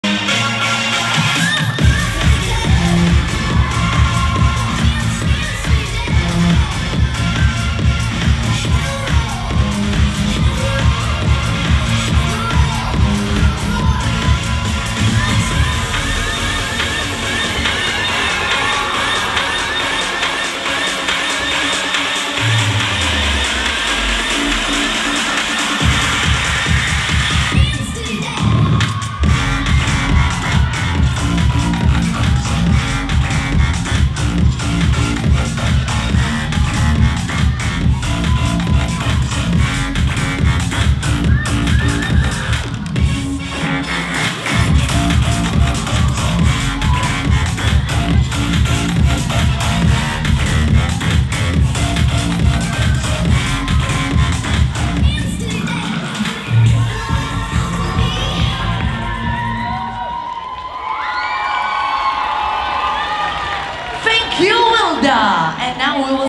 CHOMUTOV 2022 ME IDO DF Solo Juniorzy 1 WILDA TOUMARILA 4M
IDO EUROPEAN DISCO DANCE AND DISCO FREESTYLE CHAMPIONSHIPS
Chomutov  Czech Republic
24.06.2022

European Championship | Disco Dance Freestyle | Solos female | Junior 1

WILDA TOUMARILA
4  miejsce
SWEDEN